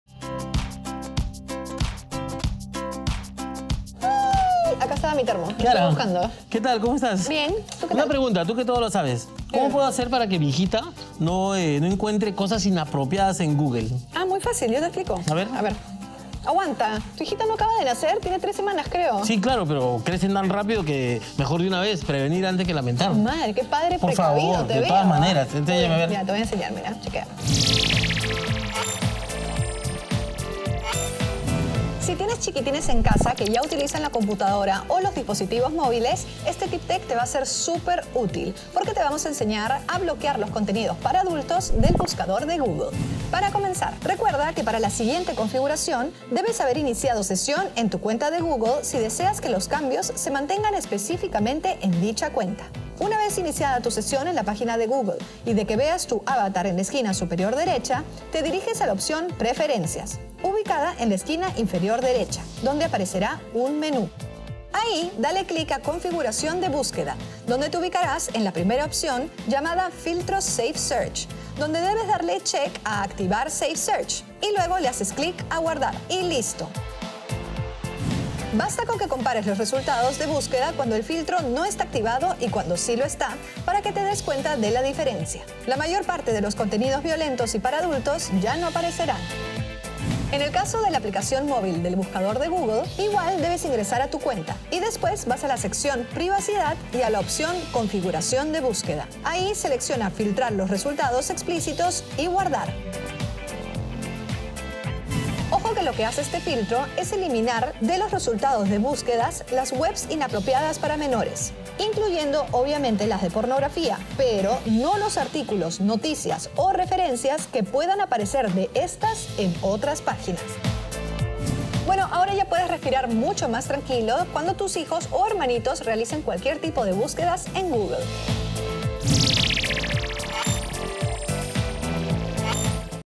Tip-TEC: Cómo bloquear el contenido para adultos en Google
Te enseñamos a bloquear el contenido para adultos en las búsquedas en Google.